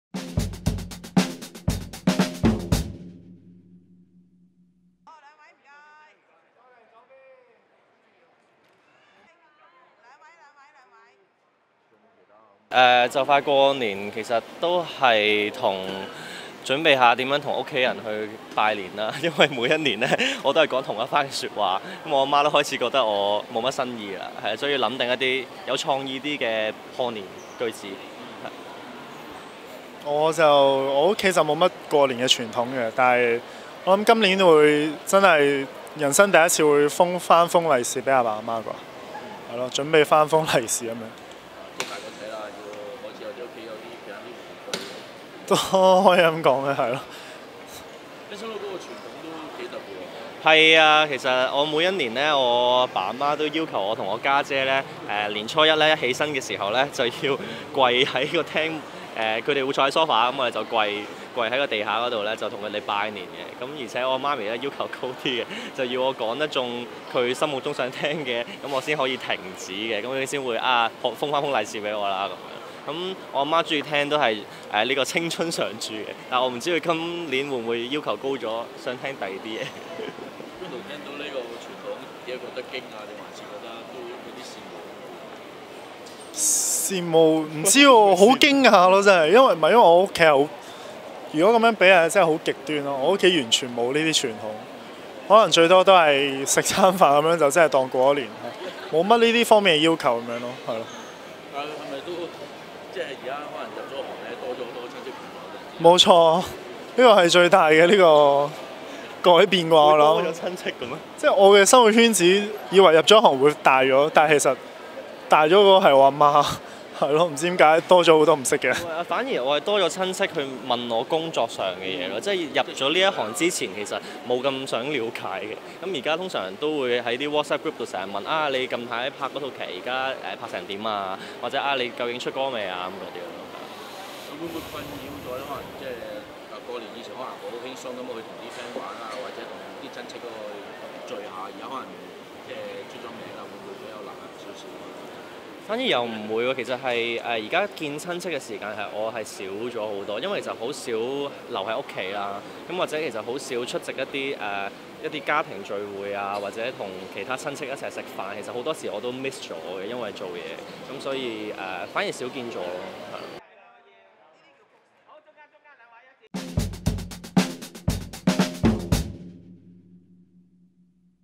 【廣東話】Anson Lo要跪地向父母拜年 姜濤：我會封返封利是畀佢哋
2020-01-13【廣東話】Anson Lo要跪地向父母拜年 姜濤：我會封返封利是畀佢哋